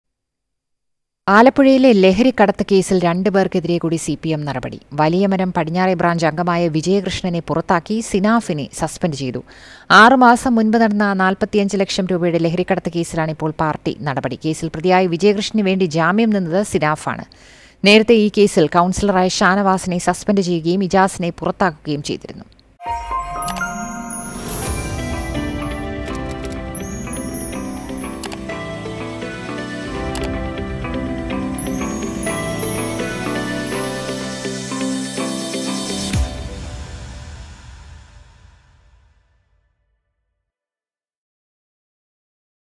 ആലപ്പുഴയില്‍ ലഹരിക്കടത്ത് കേസില്‍ രണ്ടുപേര്‍ക്ക് എതിരെ സിപിഎം നടപടി
ആലപ്പുഴയിലെ ലഹരിക്കടത്ത് കേസില്‍ ആരോപണം നേരിട്ടവര്‍ക്ക് എതിരെ സിപിഎം നടപടി എടുത്തു
one member suspended and another one dismissed alappuzha cpm takes disciplinary action on drug case